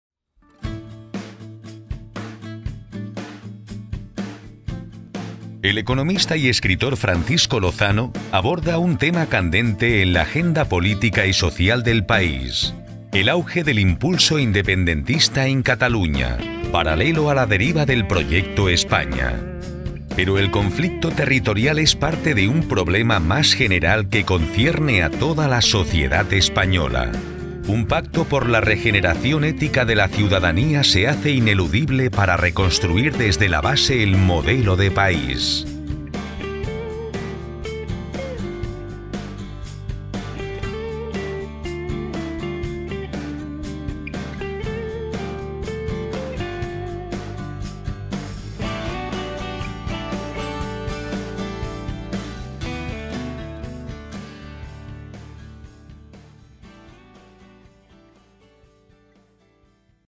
Por la Vía de la Regeneración (Booktrailer) - Editorial Círculo Rojo
BALANZAS y CUENTAS PENDIENTES. DECEPCIONES y EMOCIONES en
liza. ¿CATALUNYA con o frente a ESPAÑA?
¿NOS ESTAMOS HACIENDO LAS PREGUNTAS CORRECTAS?
Tras un largo ciclo de prosperidad económica repleto de excesos, que desactivó controles
preventivos, alentó a espabilados, encubrió a corruptos y sedujo a buena parte de
la ciudadanía, España se enfrenta a una TRIPLE DERIVA: la de su modelo económico,
la de sus instituciones y la de su modelo de Estado. La pérdida de certezas y la
dureza de los ajustes aplicados sobre el Estado del Bienestar ―el único patrimonio real
de las clases medias y bajas―, ha generado primero indignación y después desafección
hacia lo que el PROYECTO ESPAÑA, de carácter continuísta, puede aportar. El pulso
soberanista que ha irrumpido con fuerza inusitada en CATALUNYA se ha alimentado
en buena parte de esta desafección hasta lograr una masa crítica significativa para la
que las demandas económicas han dejado ya paso a lo emocional. El dilema para muchos
entre España y Catalunya se resolverá en beneficio de aquel proyecto que sea capaz
de convencer en su propuesta de un futuro con expectativas.
Hay tareas urgentes para tratar de enmendar el problema: revisar el modelo de financiación
autonómico, reordenar las competencias, dotar de sentido al Senado ―como corresponde
a la aspiración federalista surgida de la primera transición― y, cómo no,
abordar la colisión entre la aspiración a la equidad interterritorial y la pervivencia de
cuestionables asimetrías fiscales.
Pero ni siquiera todas estas reformas, de alcance constitucional, bastarían para encarrilar
el proyecto España. POR LA VÍA DE LA REGENERACIÓN propone un cambio
de enfoque que vaya más allá de la cuestión territorial para intentar remover a la
propia sociedad, a la escala de valores que hemos utilizado para desenvolvernos en el
ámbito doméstico y en la esfera pública. Es necesaria una revisión profunda del cómo
y del para qué del modelo educativo y poder dejar atrás el paradigma de lo material y
de lo individual y situar la ÉTICA y la defensa del BIEN COMÚN ―el gran damnificado
de esta última crisis―en la lógica de los comportamientos y actitudes de los próximos
políticos, gestores, familias e individuos.
Hay alternativas ilusionantes, que no sólo pueden ser capaces de dilucidar el dilema que
muchos catalanes tienen entre un proyecto continuista y otro rupturista, sino también
de dilucidar el conflicto que el conjunto de la sociedad española tiene consigo misma.